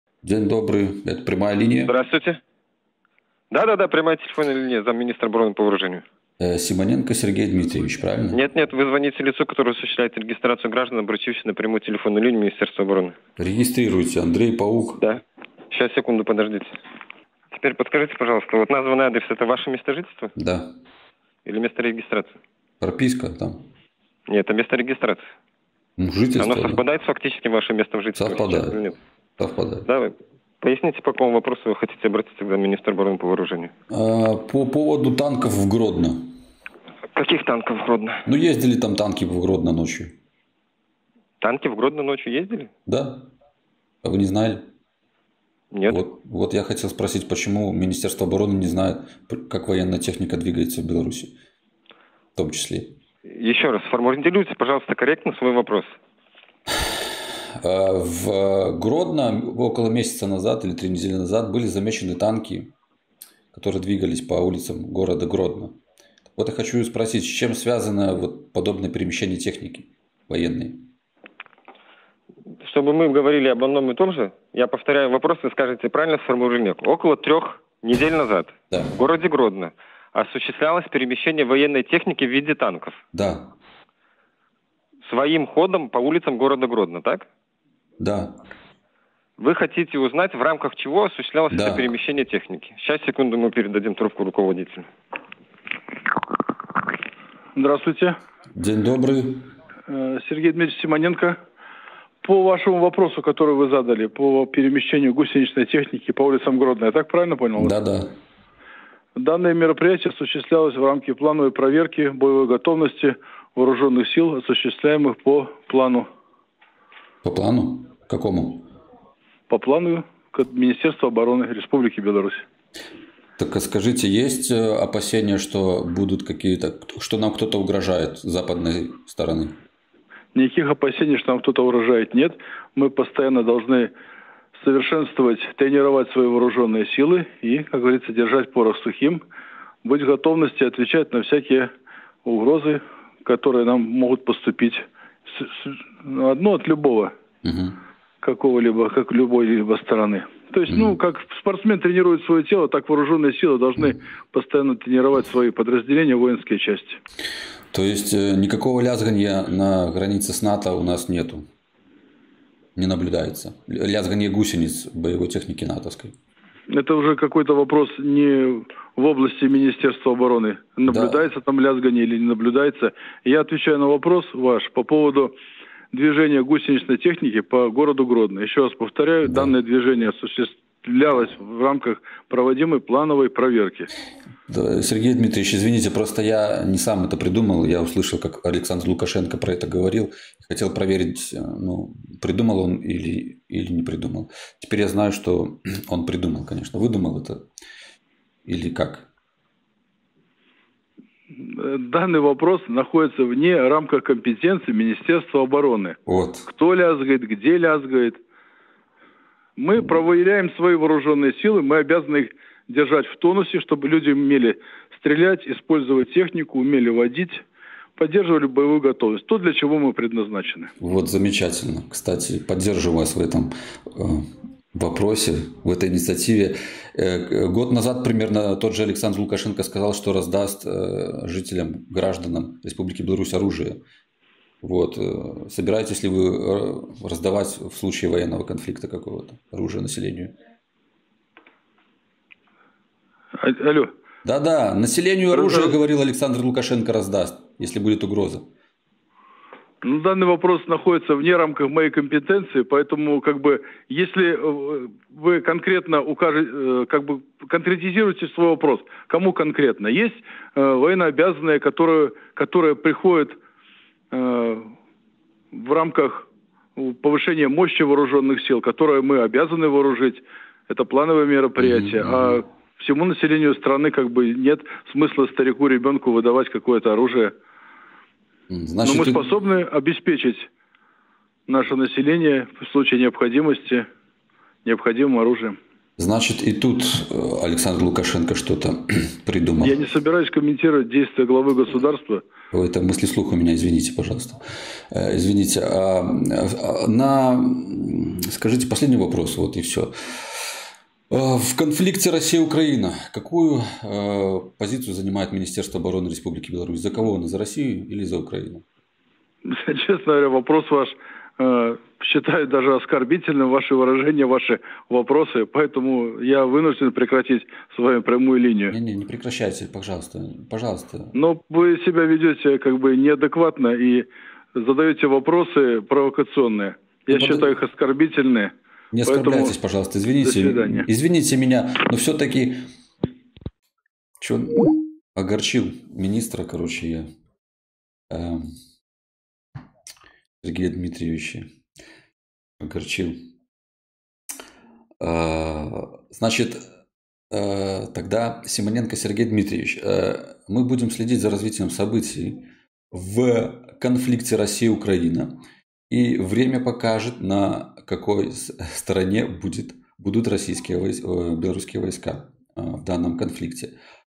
За Россию или за Украину? Симоненко Сергей оскорбился.
На чьей стороне беларуская армия?